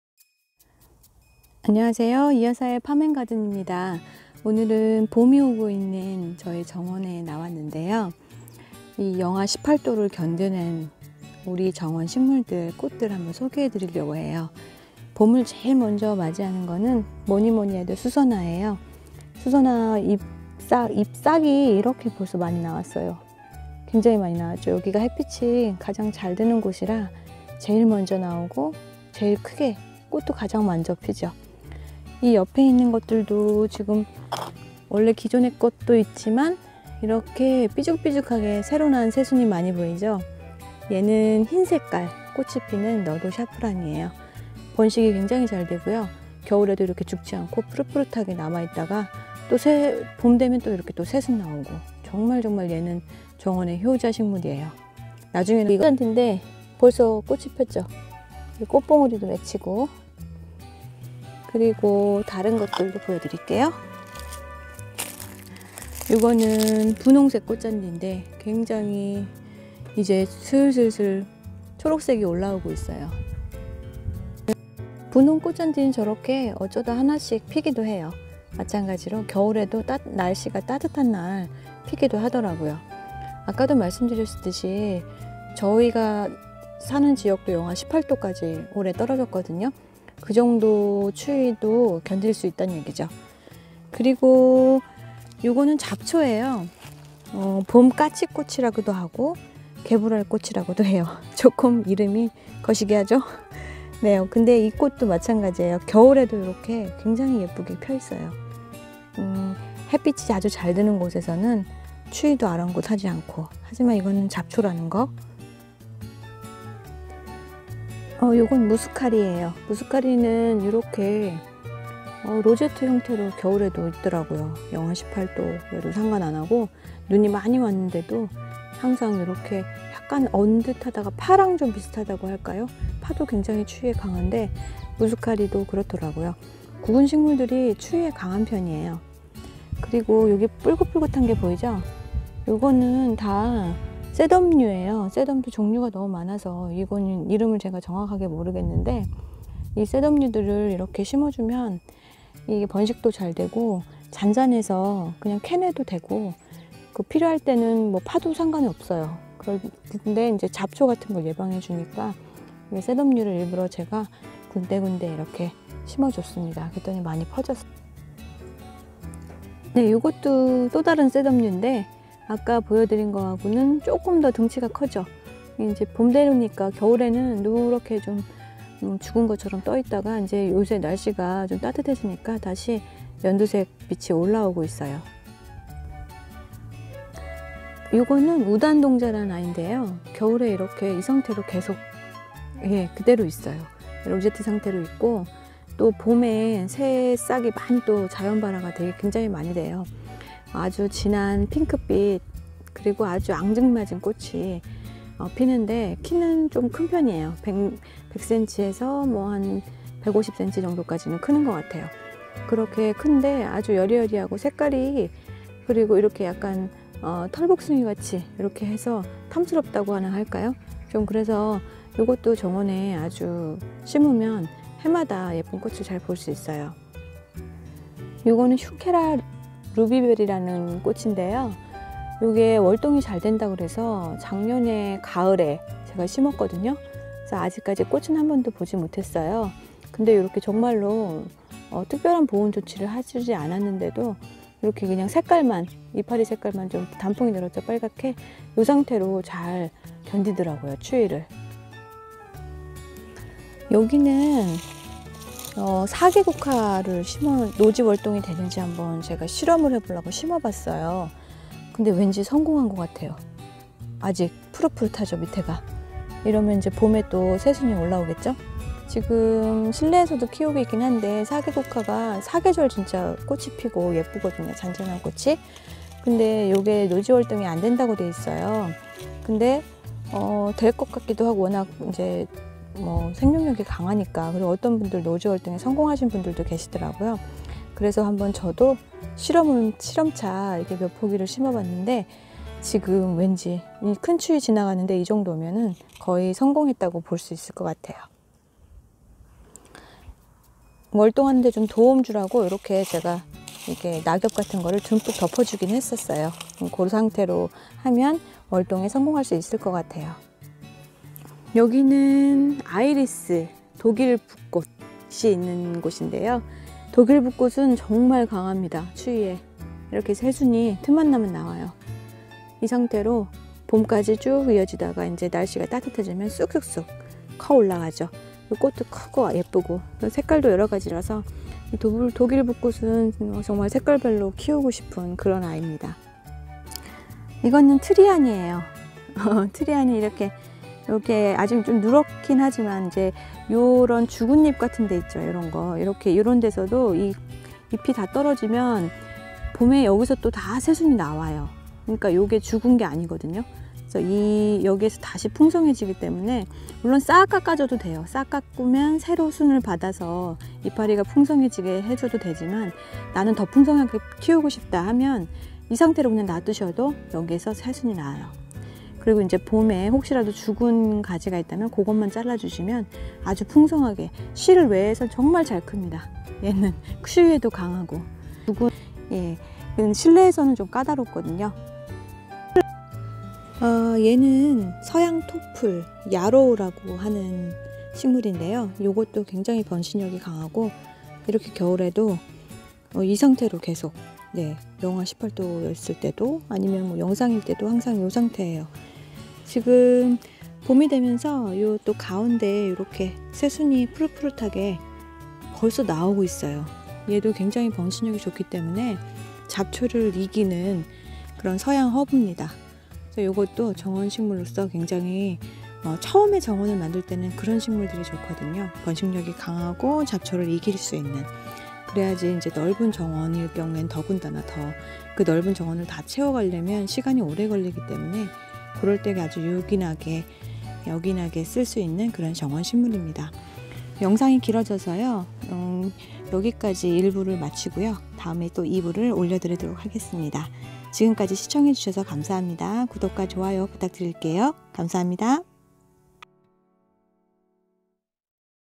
정원에 뭘 심을지 고민 중? -18도에서도 살아남은 노지월동/자연번식 잘되는 식물Cold-resistant & easily germinated plants in the garden
정원에 뭘 심을지 고민된다면?
노지월동 잘되고 번식 잘되는 정원식물 소개합니다. 정원을 처음 만드려는 가드너에게 추천합니다!